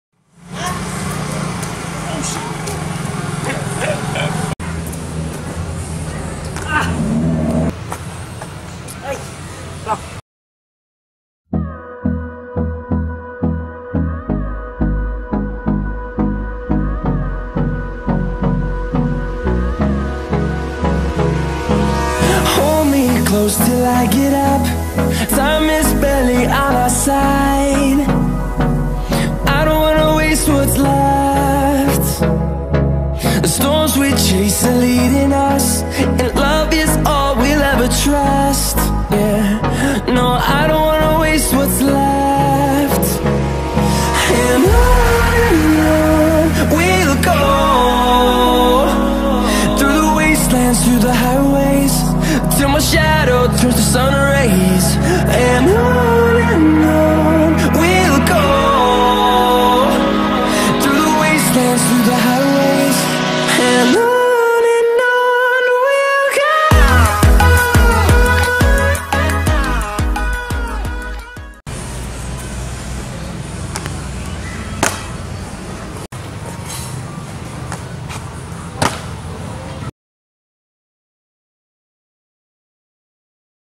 Parkour Tanah Bumbu - Short clip #2
Lokasi : Taman Edukasi Simpang Empat Tanah Bumbu

"jangan menyerah pada sesuatu, istirahat boleh. berhenti sejenak. lakukan lagi besok "